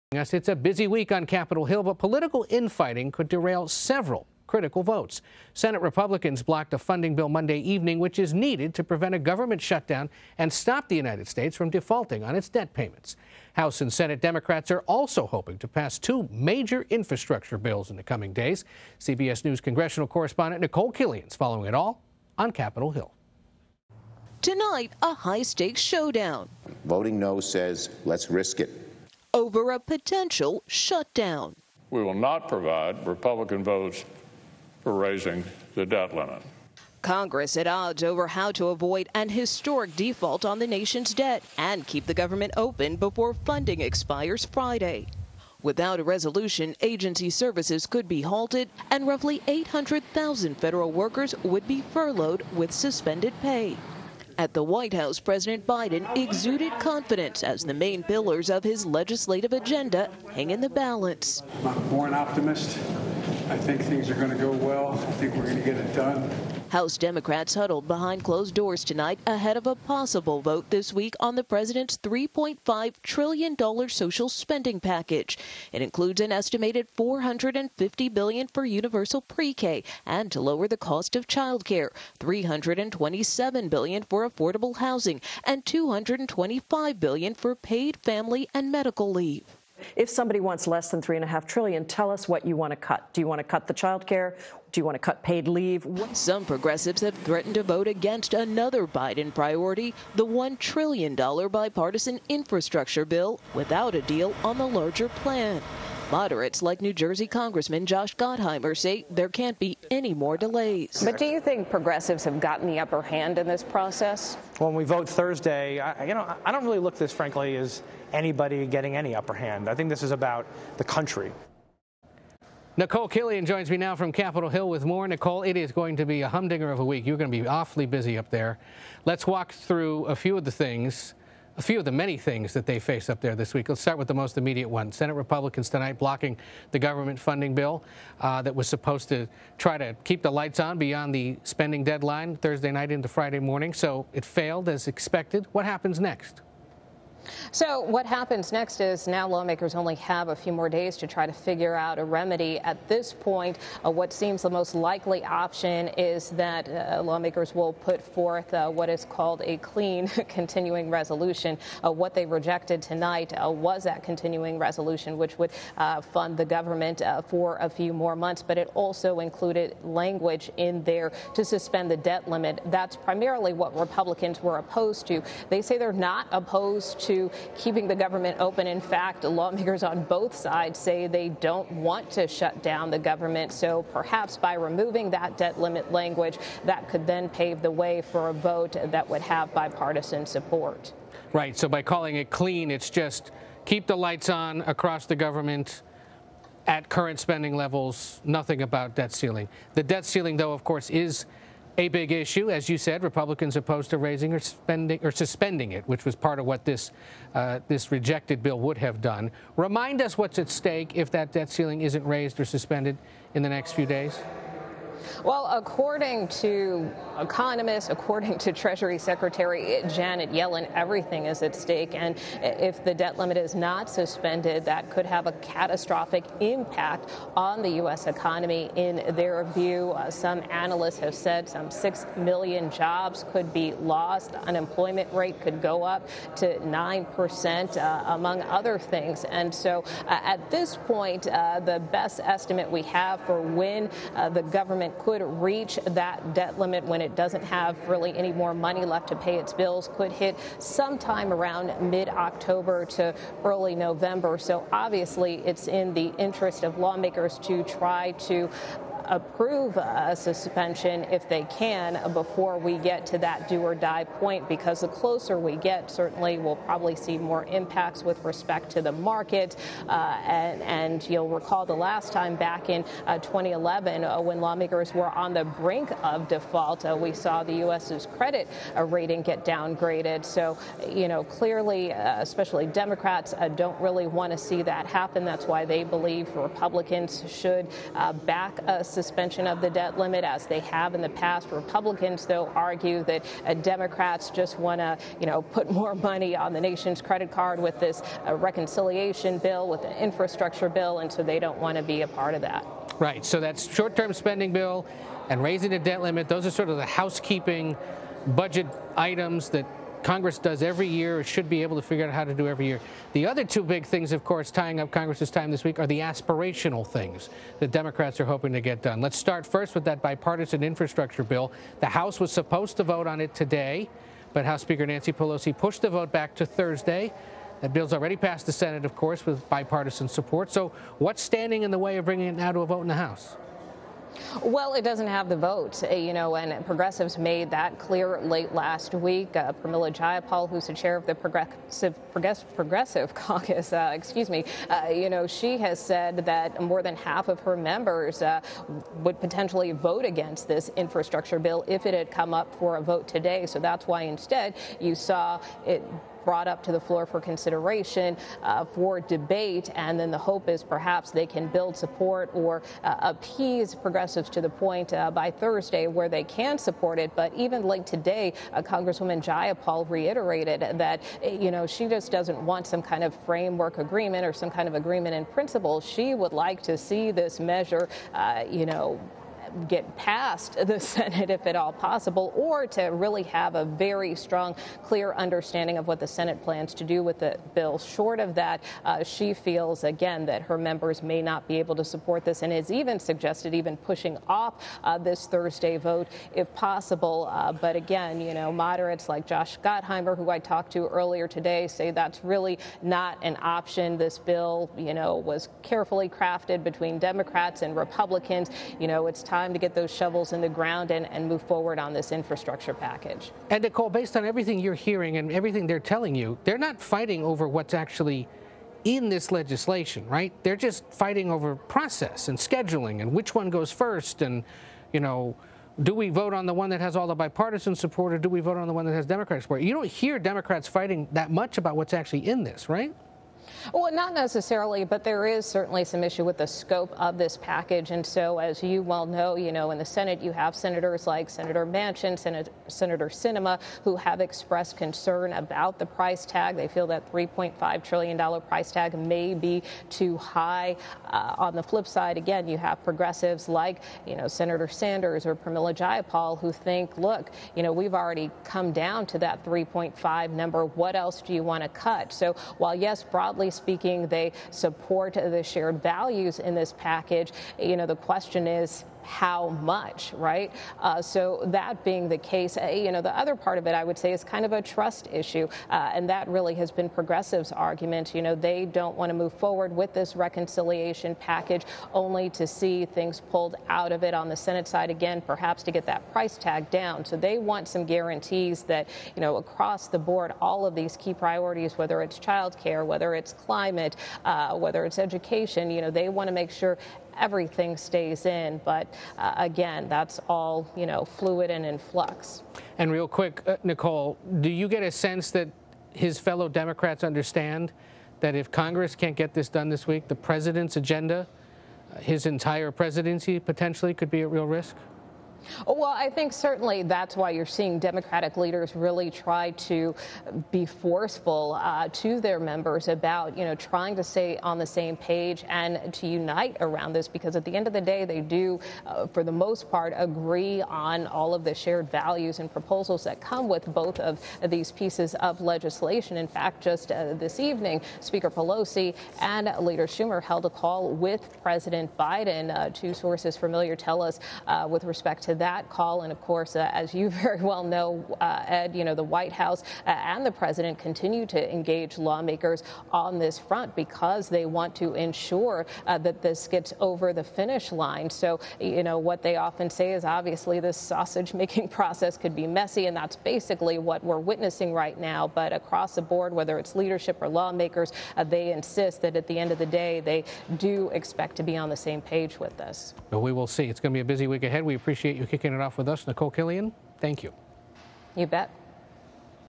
Senate Republicans block government funding bill, setting up a possible government shutdown
Senate Republicans on Monday evening blocked a bill that would temporarily prevent the federal government from shutting down and allow the United States to continue paying its debts. CBS News congressional correspondent Nikole Killion joins CBS News' Ed O'Keefe to discuss the next moves in trying to prevent a government shutdown, and where negotiations on two major infrastructure bills stand.